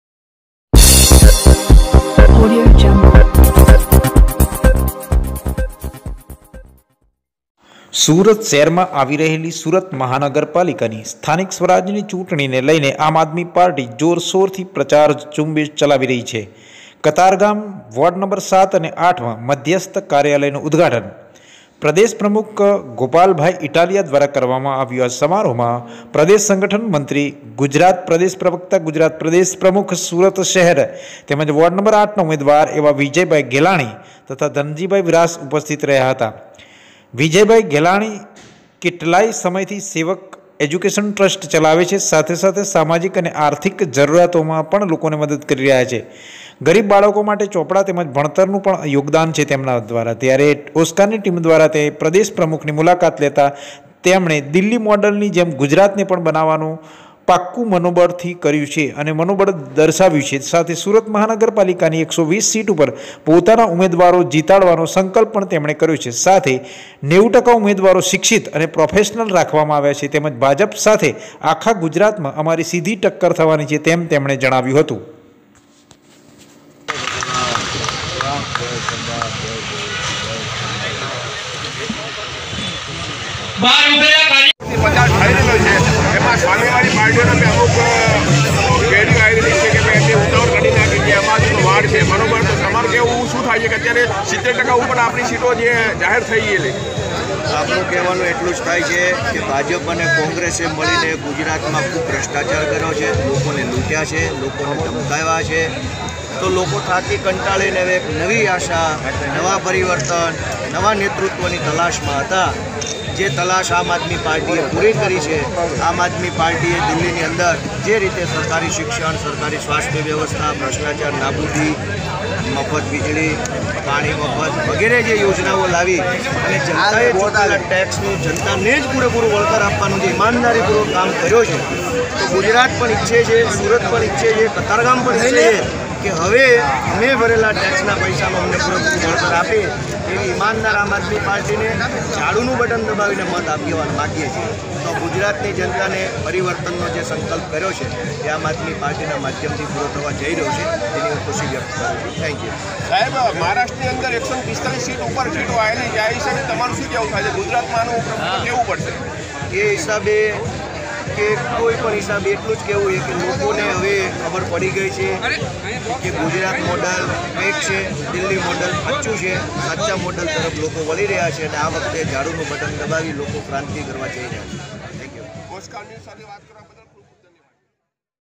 Surat કતારગામ ના વોર્ડ ન 7- 8 માં મધ્યસ્થ કાર્યાલય નું ઉદગાટન પ્રદેશ પ્રમુખ એવા ગોપાલ ભાઈ ઈટાલીયા
Oscar News Pvt. Ltd.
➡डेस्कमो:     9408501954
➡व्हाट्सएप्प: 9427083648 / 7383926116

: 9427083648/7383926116